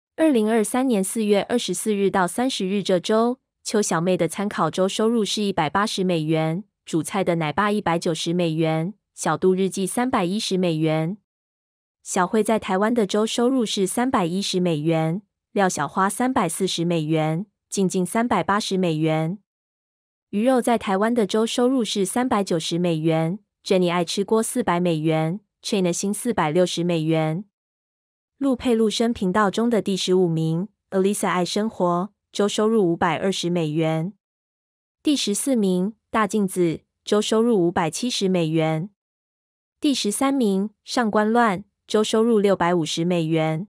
陸配陸生頻道2023.4.24-30收入排行13到24名，上官亂，大静子，Alisa愛生活，Cianna欣，jennie愛吃鍋，鱼肉在台湾，靜靜，廖小花，小卉在台灣，小杜日記，煮菜的奶爸，秋小妹
按照每千次觀看2.5美元統一計價。收入區間按照每千次觀看1.5美元到3.5美元計價。粉絲活躍度是用日均觀看次數除以訂閲人數。
第1名到第12名分別是：我是黨黨，何新瑶，我們倆 ，村花嫁台灣，摩沙淺淺，成都妹子在台灣，新住民花花，胡辣湯姐姐，圓臉麥麥，Here UU來了，小夢的田園生活，Lemon新生活
24個陸配陸生頻道4月24日-4月30日總流量比上週下降8.7%。